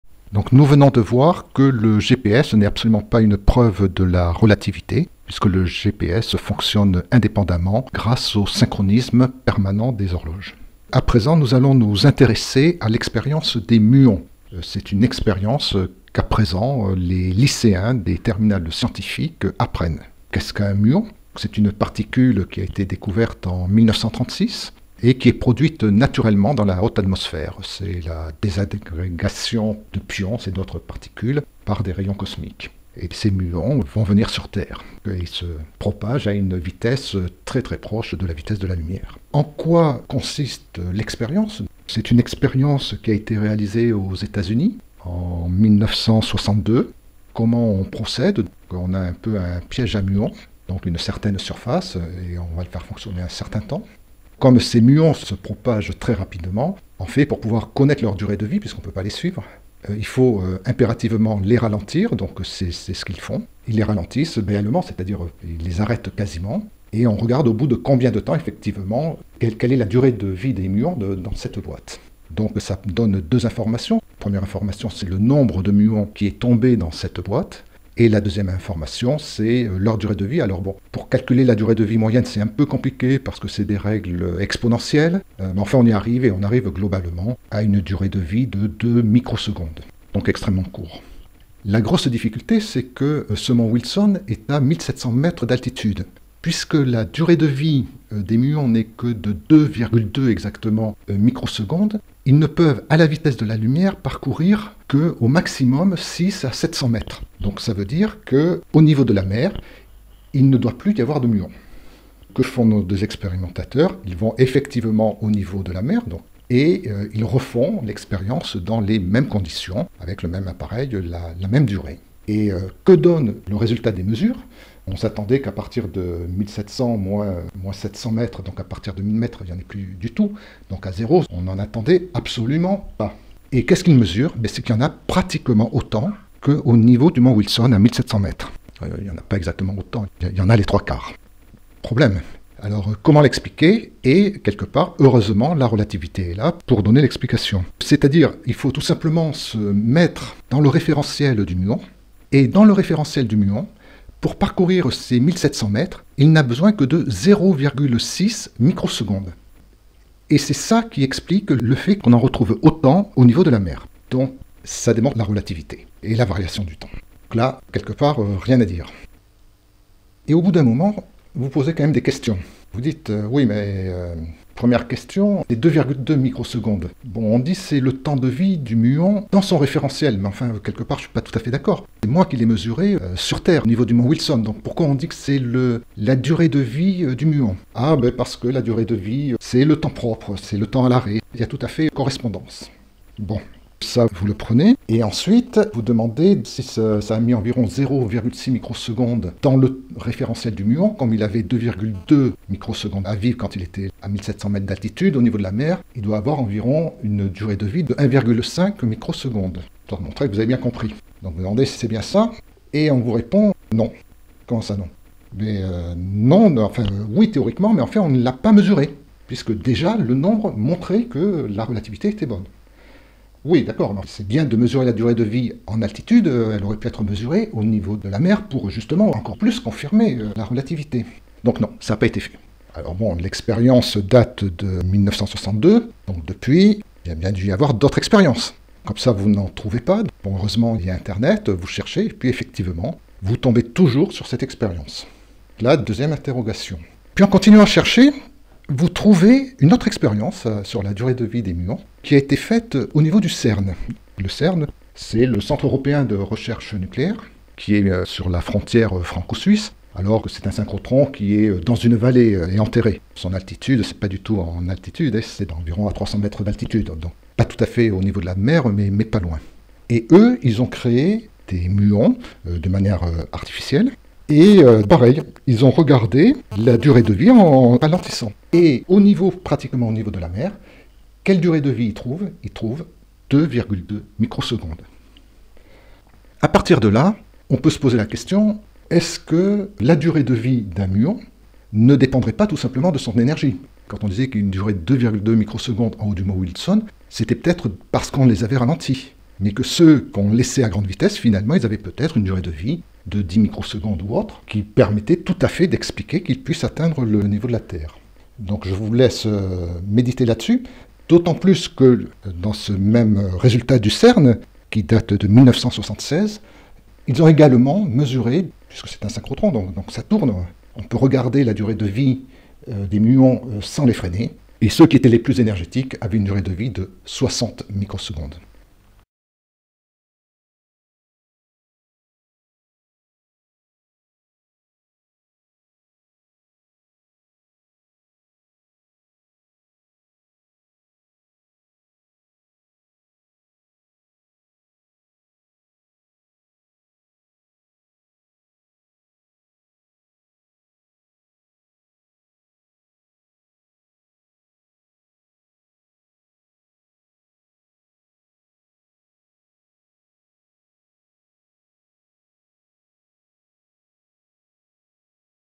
7 - La durée de vie du Muon n'est PAS une preuve de la Relativité !
Analyse critique des mesures réalisées sur la durée du vie des muons comme preuve de la Relativité. Cours de Physique Terminale S Scientifique.
00:00       Présentation
00:38       L'expérience de 1962
02:24       L'explication relativiste
03:56       L'expérience du CERN
04:45       L'explication néo-newtonienne